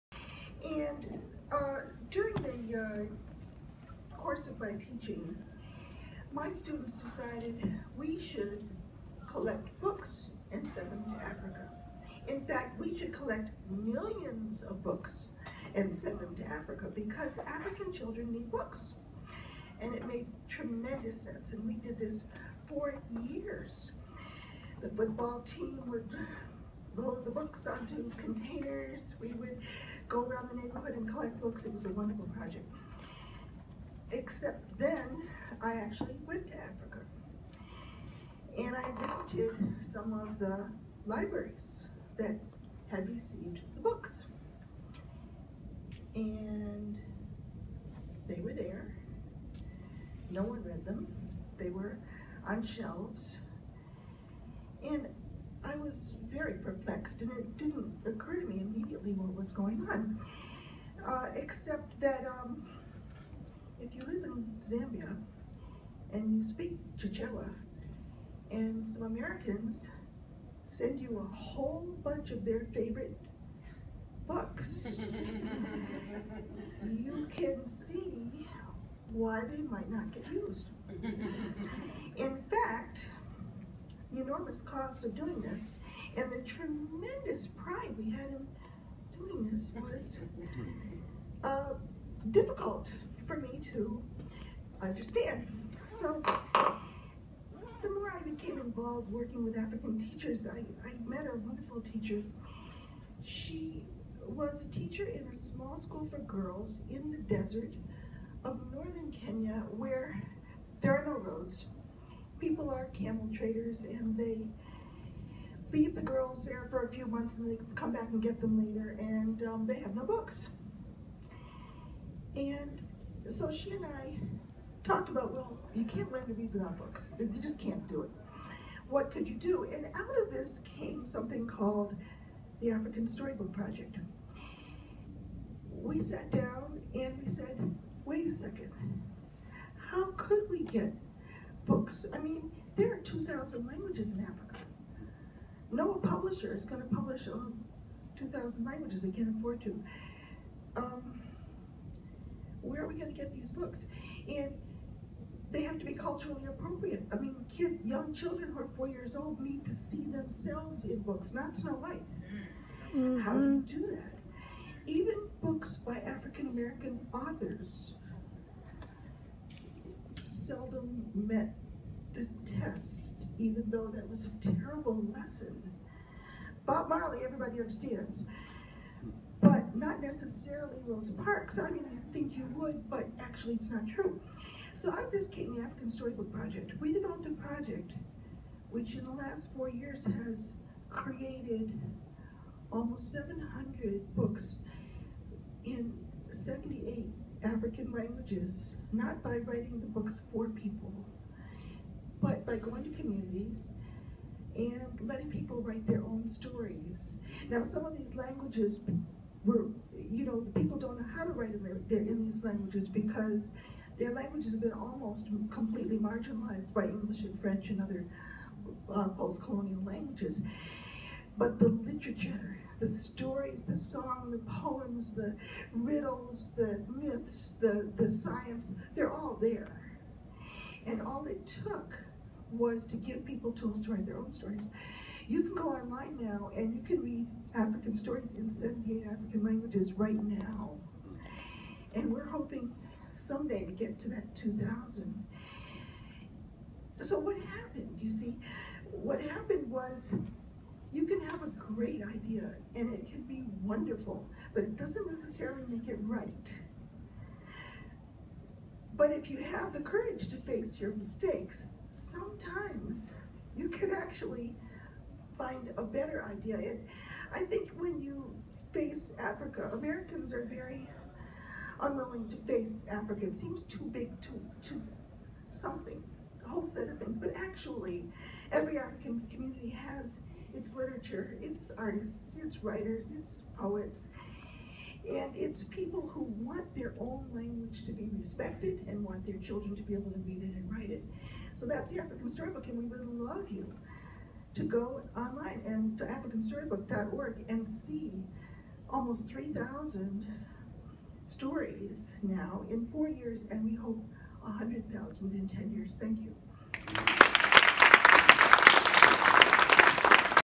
Judith Shares Her Story of Grassroots Change
Stories of Grassroots Change Event at HUBWeek 2016